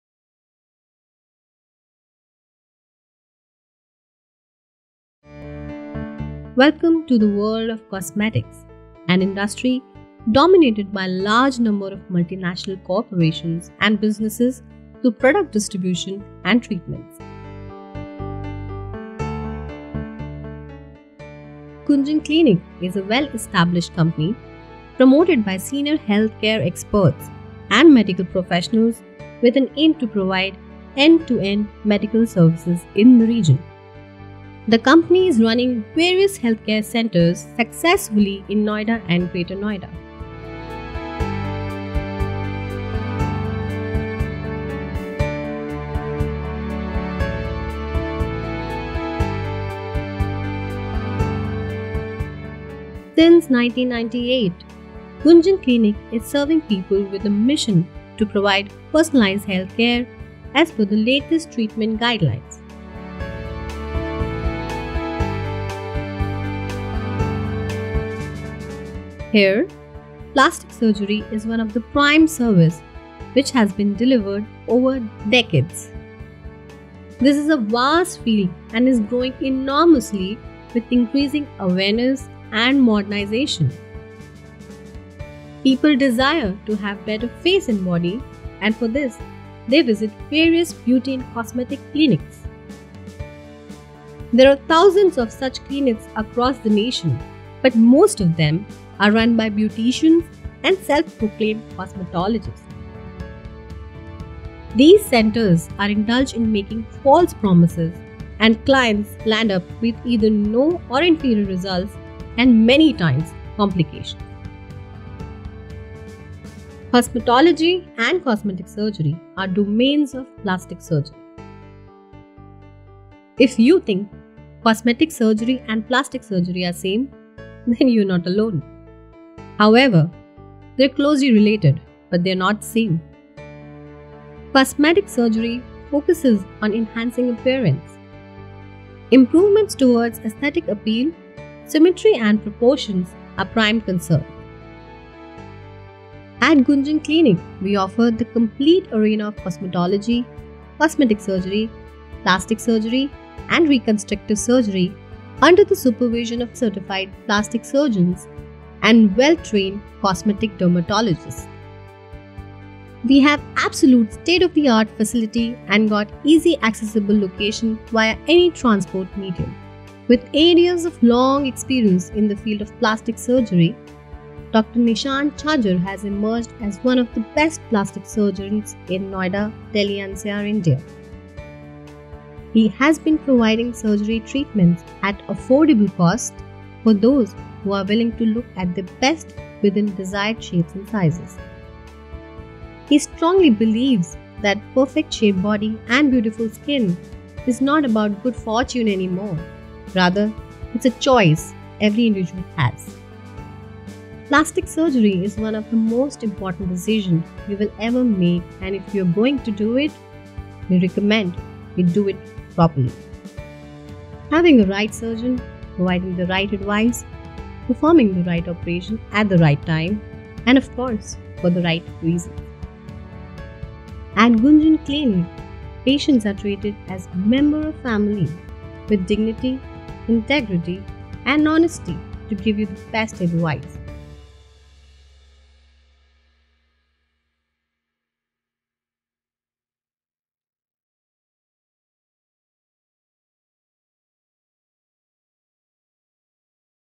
Dr. Nishant Chhajer | Cosmetic & Surgery | Corporate Film - HNI Times
A New corporate film is being launched to address cosmetic & surgical treatments by Dr. Nishant Chhajer.